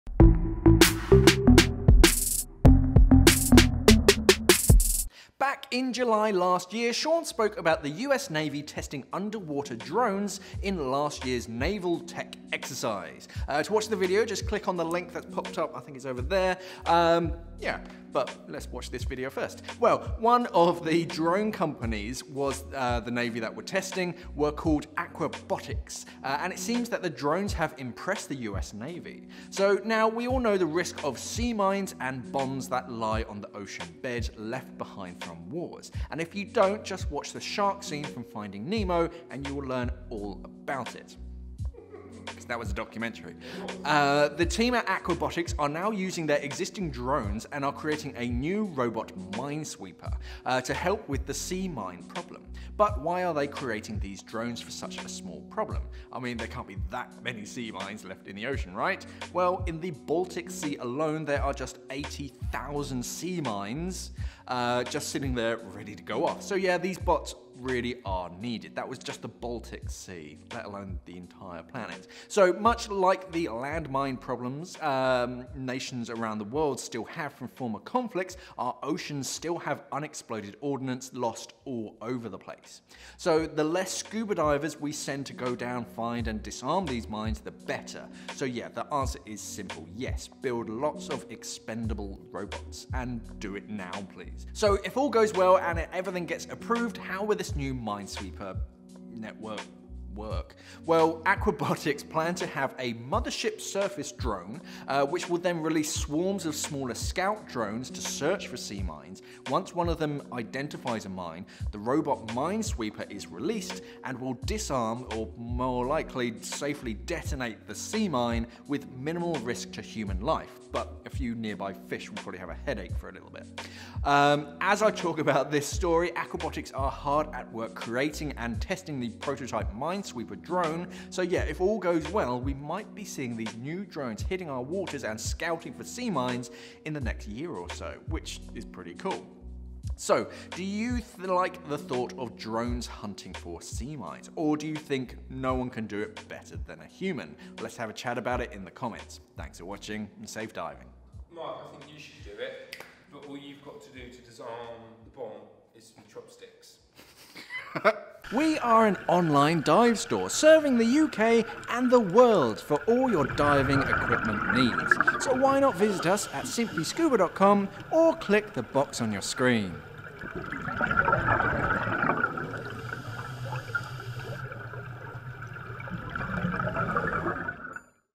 Daily Scuba News - Are robots the future to destroying sea mines?
Back in July last year Shaun spoke about the U.S. Navy testing underwater drones in last years navel tech exercise, to watch the video just click on the link that has just popped up. Well, one of the drone companies the Navy were testing is called Aquabotix and it seems that the drones have impressed the US Navy. Now, we all know the risk of sea mines and bombs that lie on the ocean bed left behind from wars, and if you don’t just watch the shark scene from Finding Nemo then you will know all about it.

The team at Aquabotix are now using their existing drones and are creating new robot minesweepers to help with the sea mine problem. But why are they creating these drones for such a small problem? I mean there can’t be that many sea mines left in the ocean right? Well in the Baltic Sea alone there are 80,000 sea mines just sitting there ready to go off… so yeah these bots really are needed. Much like the landmine problems nations around the world still, have from former conflicts our oceans still have unexploded ordnance lost all over the place.

Sources

Team
Presenter(s): Mark Newman
Writer(s): Shaun Johnson & Mark Newman
Editor(s): Shaun Johnson